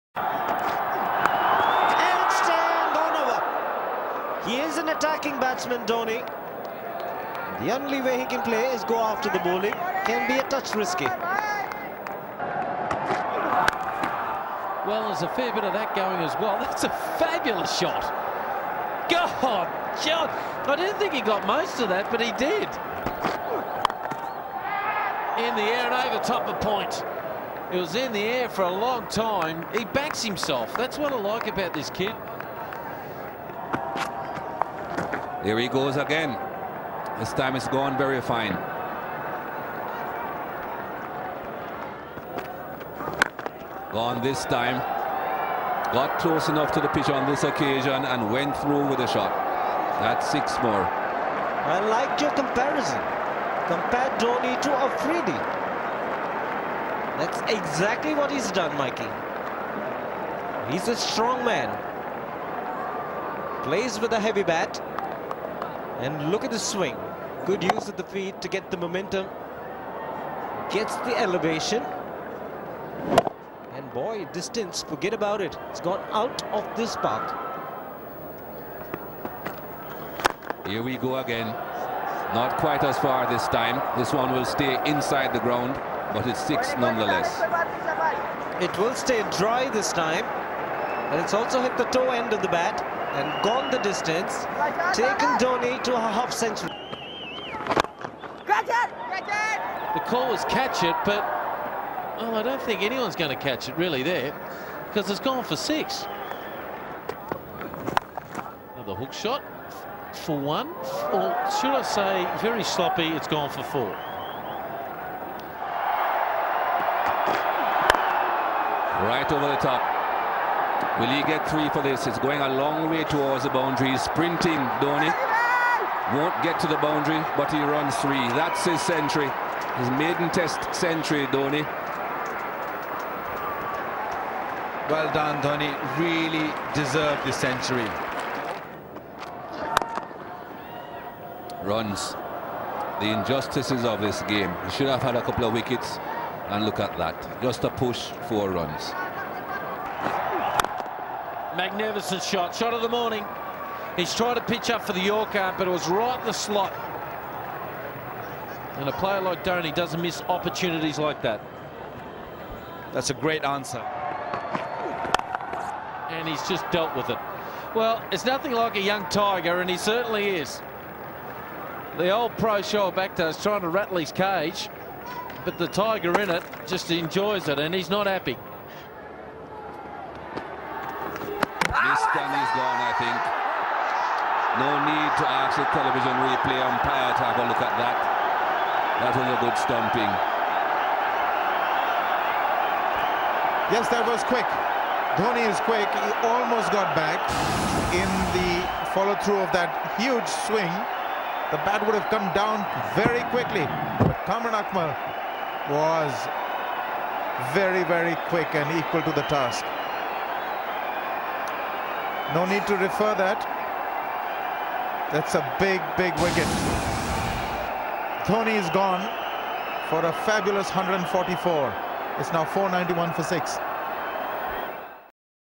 M.S Dhoni - First Test Century - 148 vs Pakistan 2nd Test 2006 @ Faisalabad
That Mahendra Singh Dhoni shredded a bowling attack is no surprise; that he did so in just his fourth Test against the ferocity of Shoaib Akhtar, with his side skidding into disaster-zone, was what caught Pakistan unaware. Apart from a brief burst after lunch, when every batsman looked intimidated, Shoaib too has been as ineffectual as every other bowler in this series. And the sense of frustration at having to do what he calls "donkey work" on such unforgiving pitches finally spilled over this morning in an eventful over to the unflappable Mahendra Singh Dhoni. After a magnificent straight-drive and a casual slash over slip, Shoaib was fuming. The short ball from round the wicket was duly unleashed, only to be dismissively pulled to the midwicket fence. The beamer that followed cast him in even poorer light and it was no surprise when Younis Khan hauled him off after just three overs.